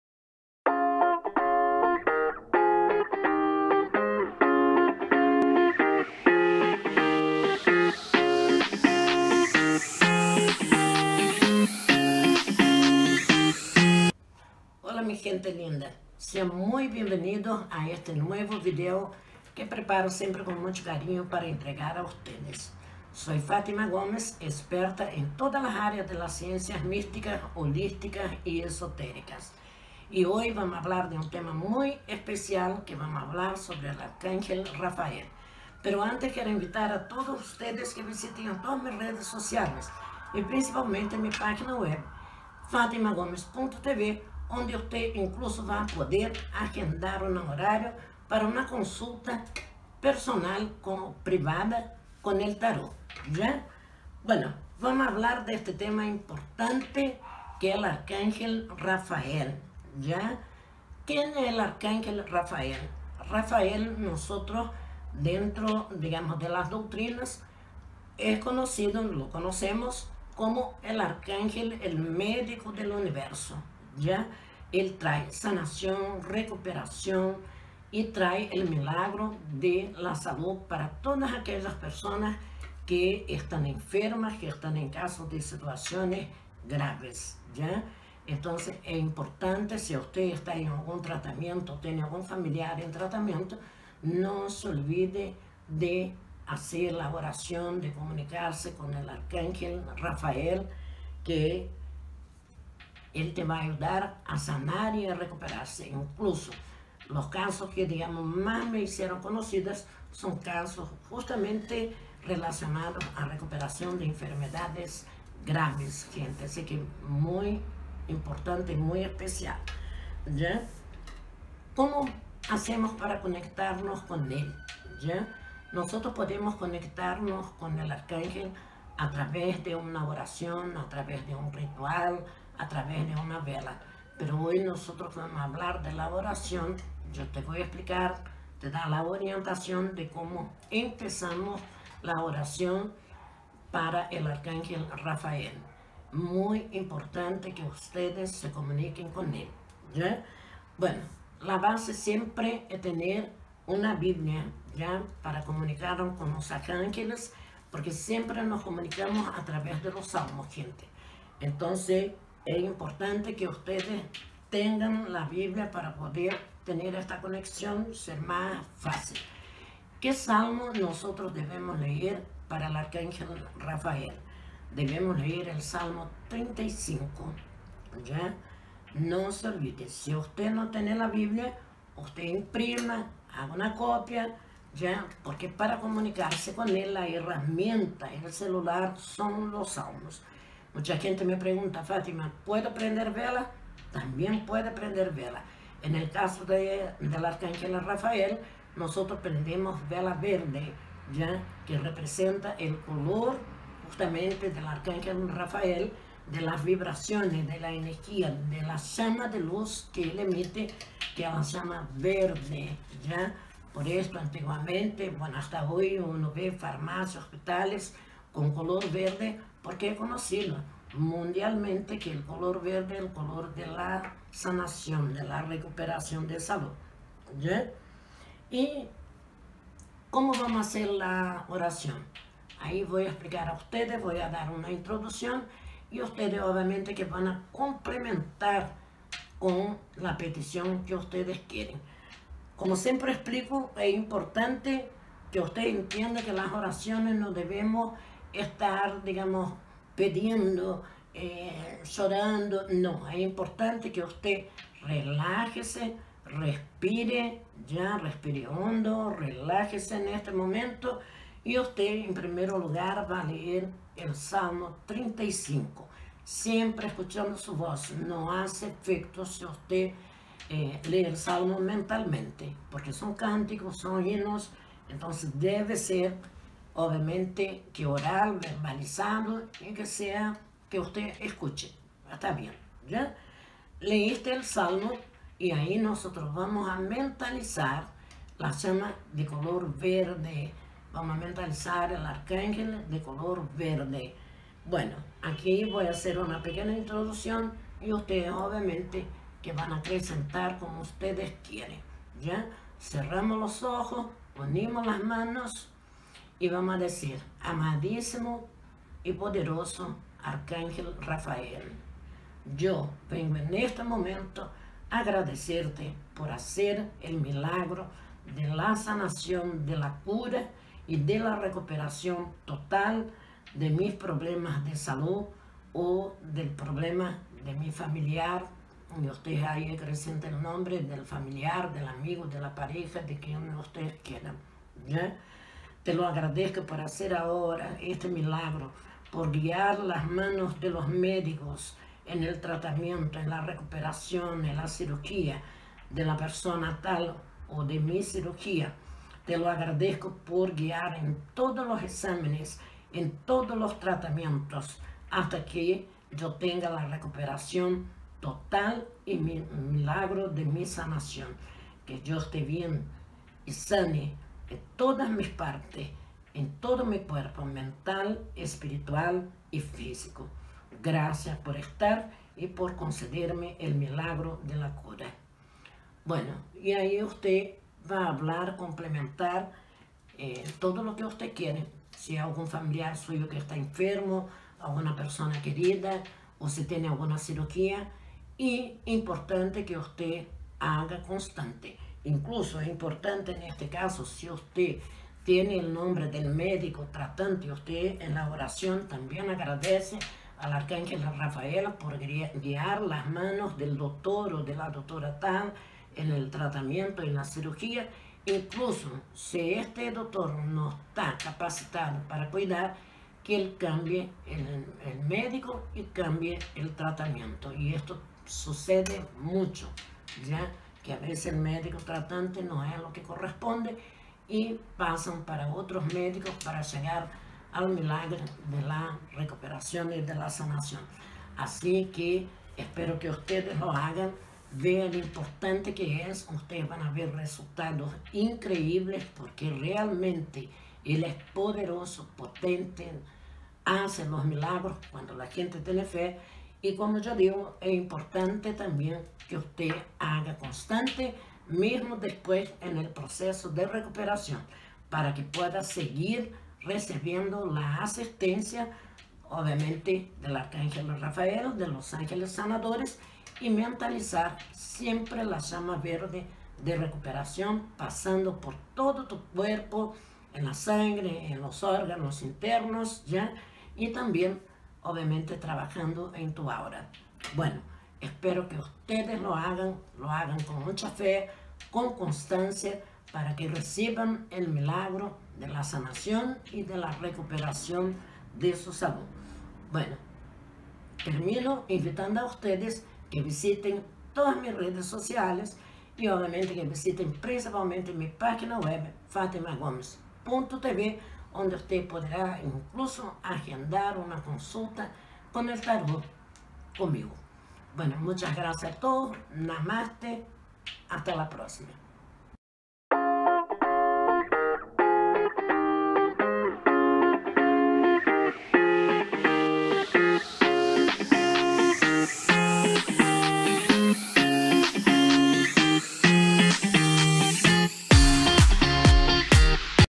Poderosa Oración para la SANACIÓN con el Arcángel Rafael
Les entrego una Poderosa Oración con el Arcángel Rafael, de como ustedes deben empezar a realizar para oración, el salmo correspondiente al Arcángel Rafael y para que sirve su oración...
¡Namaste!!